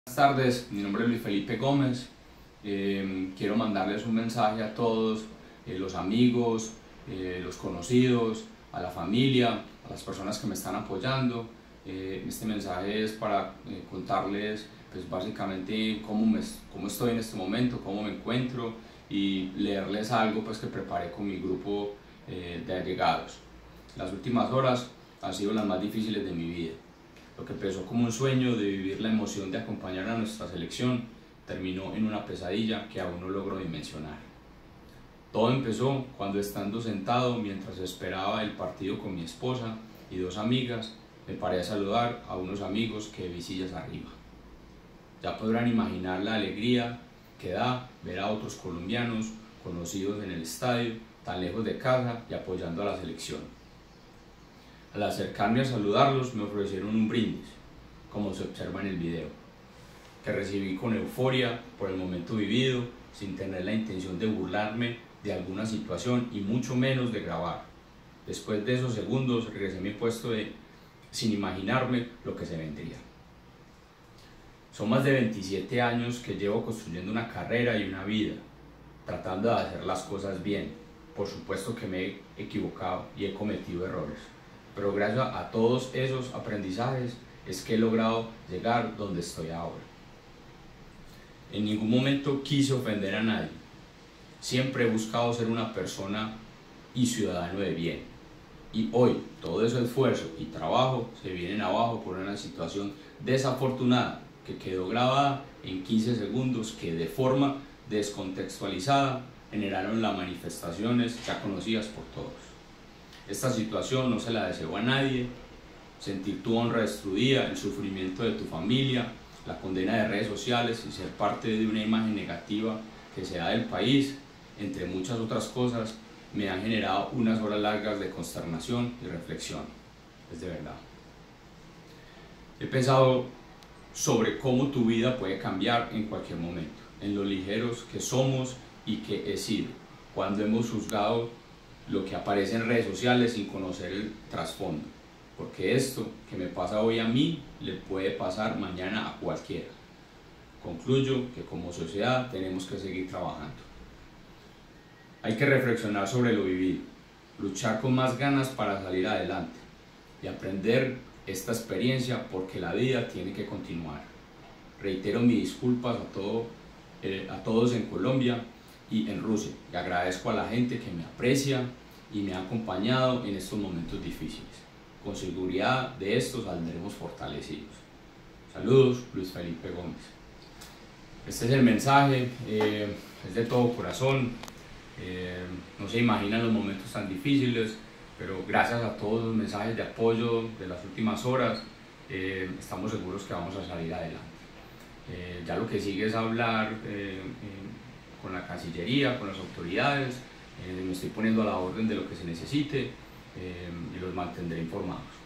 "Las últimas horas han sido las más difíciles de mi vida": Luis Felipe Gómez
Luis Felipe Gómez, el hombre que quedó grabado en video tomando licor al interior del estadio Mordovia Arena durante el partido entre la Selección Colombia y Japón, envió un mensaje a la opinión pública.